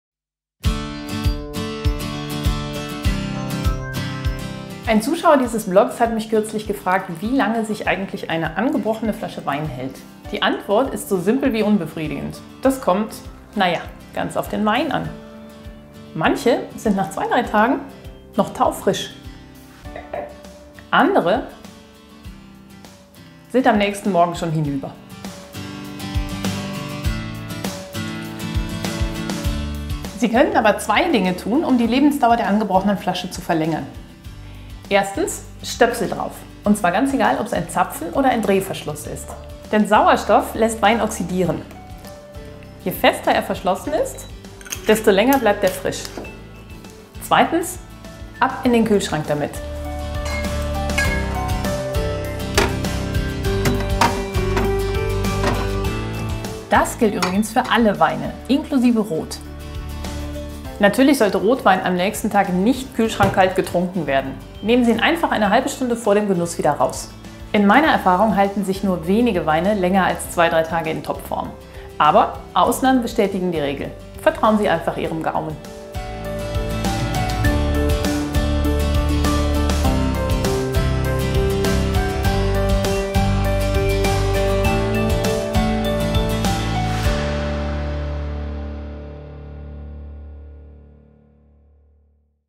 Brittas Weinlatein: So bewahren Sie eine angebrochene Flasche auf
Der Abend ist zu Ende und die Weinflasche noch halb voll? Der Kühlschrank ist Ihr bester Freund.